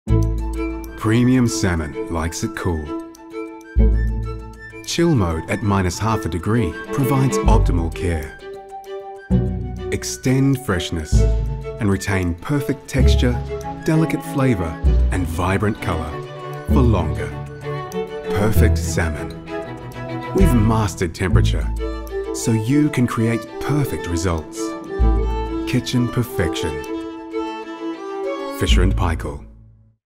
Food Care: Perfect Salmon
Premium salmon likes it cool. Master temperature with Chill Mode and retain freshness, flavour, vibrant colour and that perfect texture for longer.

Kitchen Perfection.

Explore our integrated refrigerator range with Chill Mode to extend freshness and retain perfect texture, delicate flavour and vibrant colour for longer

Browse the contemporary Fisher & Paykel refrigerator range to make a bold statement and elevate the design aesthetic of your kitchen